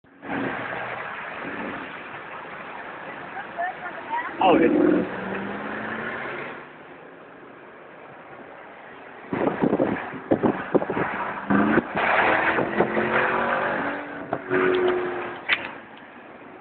Mom's GTS - parkway calabasas
Filmed from my BRABUS E500 on Parkway Calabasas